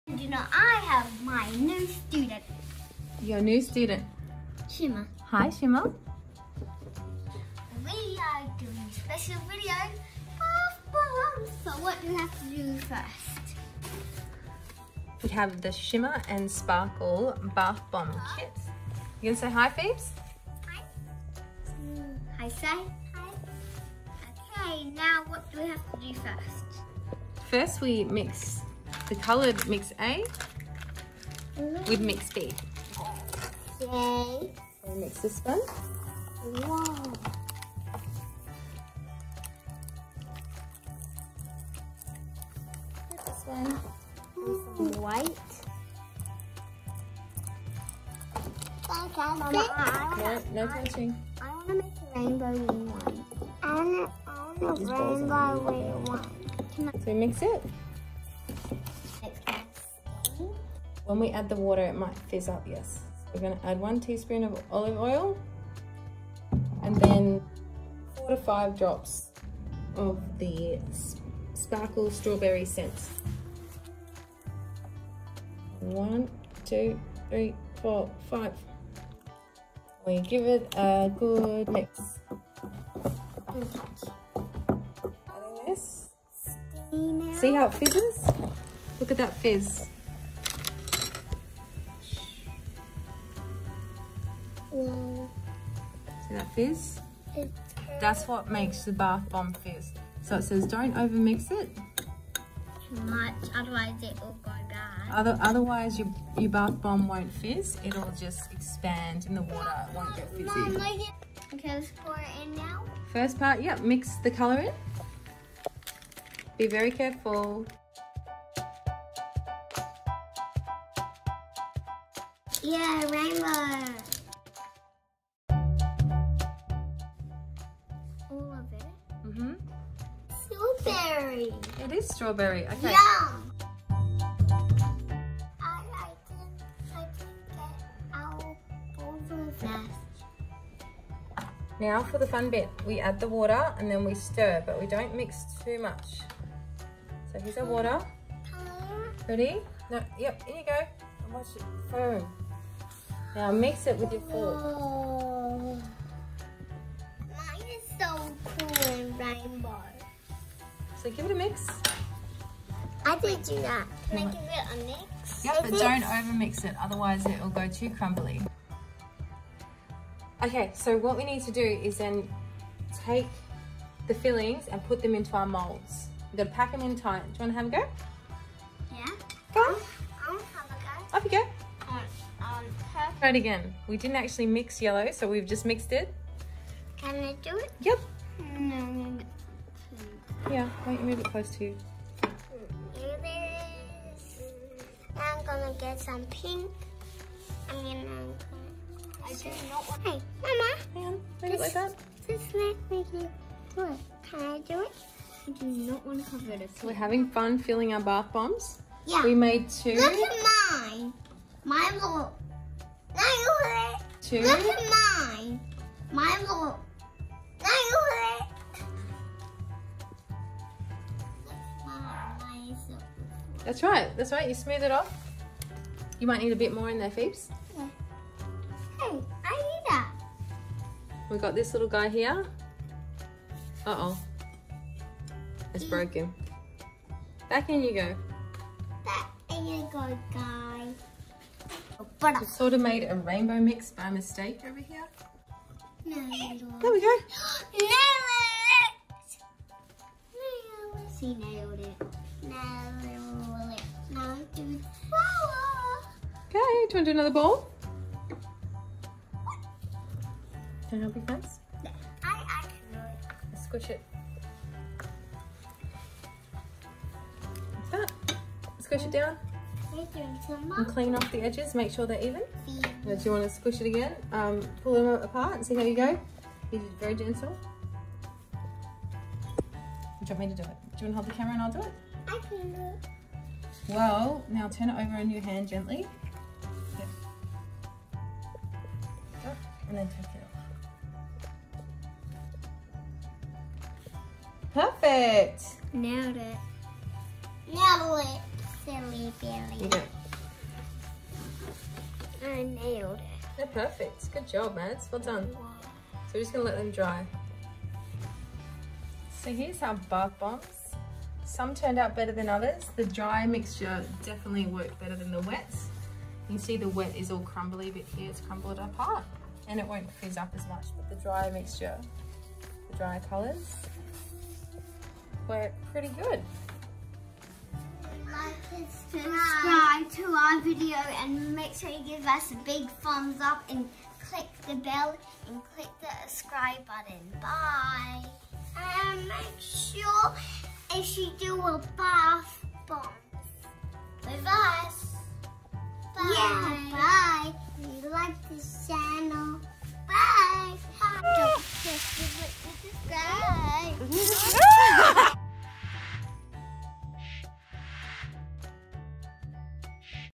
Making bath bombs with the Shimmer 'n Sparkle scented bath bomb kit! Nailed it!
The Curly girls get crafty! We make Shimmer 'n Sparkle scented bath bombs with Shimmer from Shimmer and Shine!
What colour will they make? blue? yellow? pink? RAINBOW? who knows!
If you want to know how to make bath bombs, you might want to have a little watch.
Listen out for the girl's new phrase 'Nailed it!' when they get it right.
We home you like it :)

Please note, this kit is recommended for 8 years up. The Curly Girls made this under the supervision of their parent.